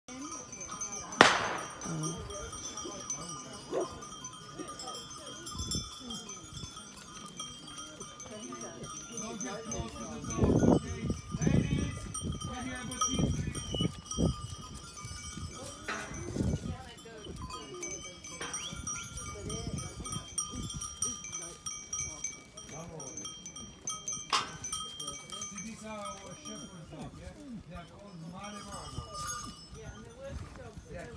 Maremma Sheepdogs Herding Sheep
Heading up the steep concrete road toward the mountain, we turn a corner to see four large white dogs—which are the Maremma Sheepdog indigenous to central Italy—leading a herd of Italian goats. The herd is varied in color: black, grey and speckled. January 1 might be a holiday in Italy but it is still a work day for these goats. Severio yells ahead that we must get out of way of their way. The three dogs following us know to avoid the sheepdogs and hide near us. It takes several minutes for the goats to meander down the road.